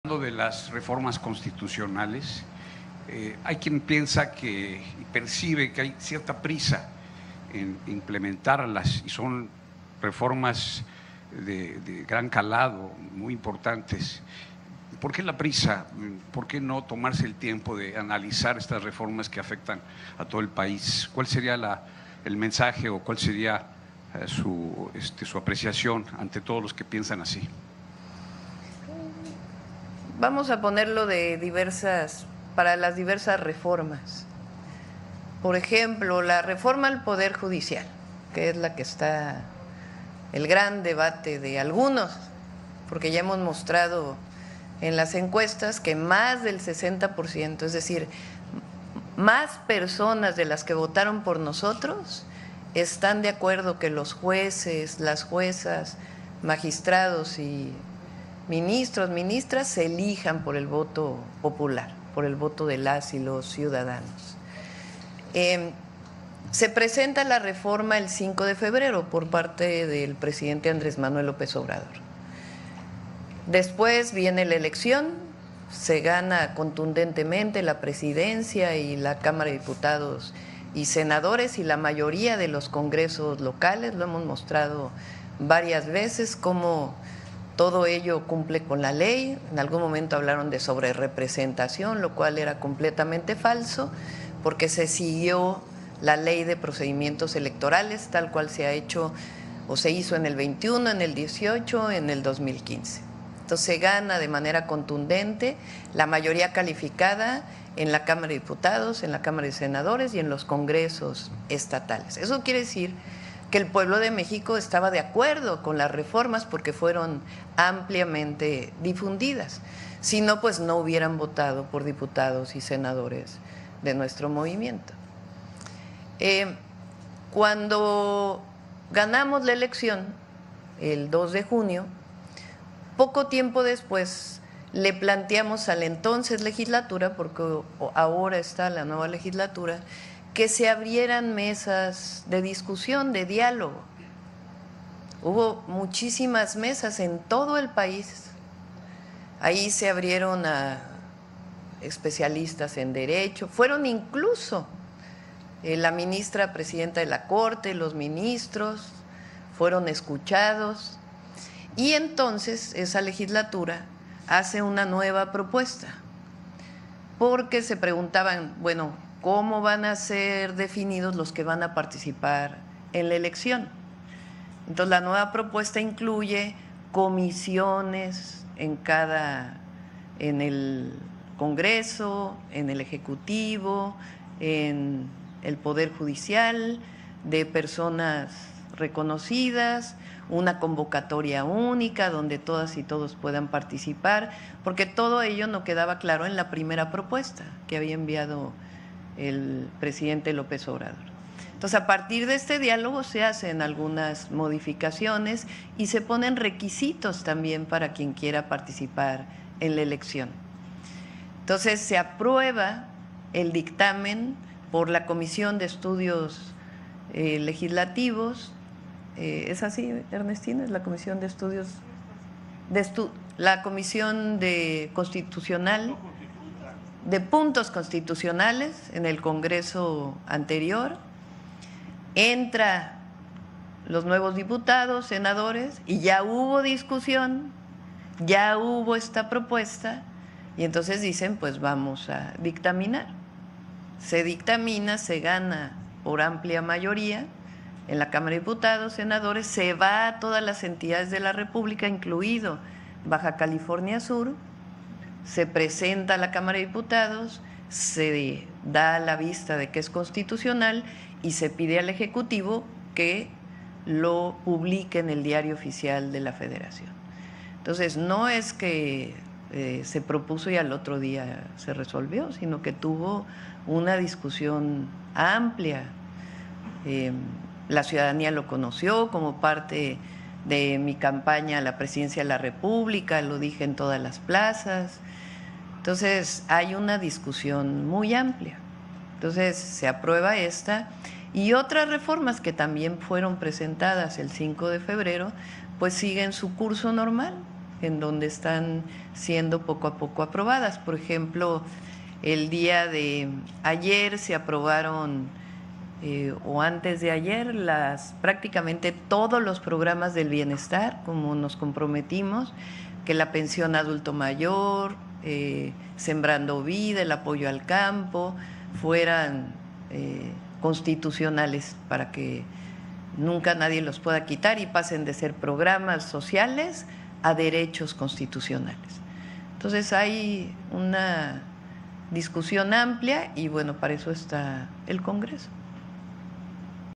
¡HAY UNA DISCUSIÓN MUY AMPLIA! Sheinbaum dice que reformas no se aprueban con prisa
Armando Figaredo le reprochó a la Dra. Sheinbaum por qué tanta prisa en implementar las reformas estructurales. La presidenta dijo que bajo el precedente de que el Pueblo de México está de acuerdo con las reformas, por ejemplo la ReformaJudicial, cuando ganaron la elección poco tiempo después le plantearon a la nueva legislatura que se abrieran mesas de diálogo a las que incluso asistieron la ministra presidenta y los ministros de la Corte, por lo que no es que estén aprobando apresuradamente